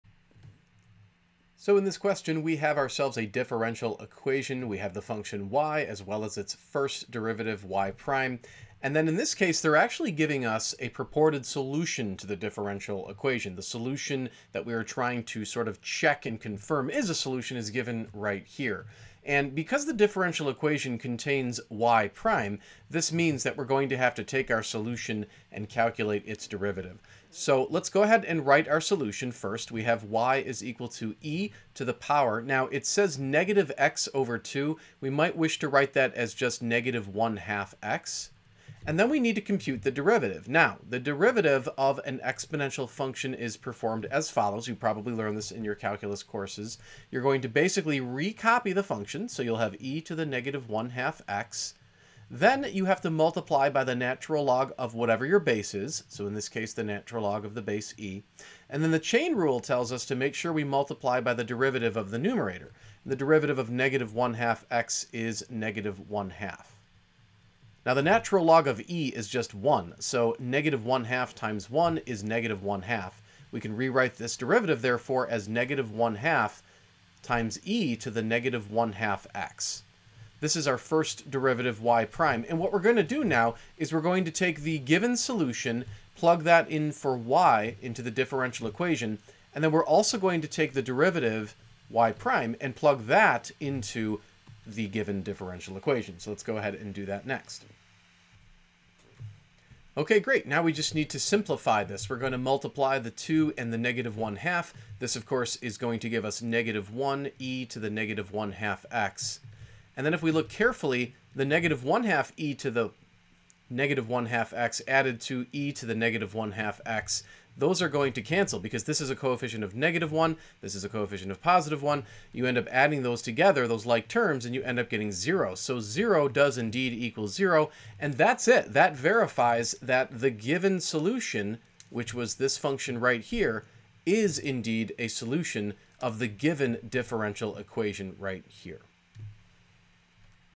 Verify that the indicated function is a solution of the differential equation
Verify that the indicated function is an explicit solution of the given differential equation. Assume an appropriate interval I of definition for each solution.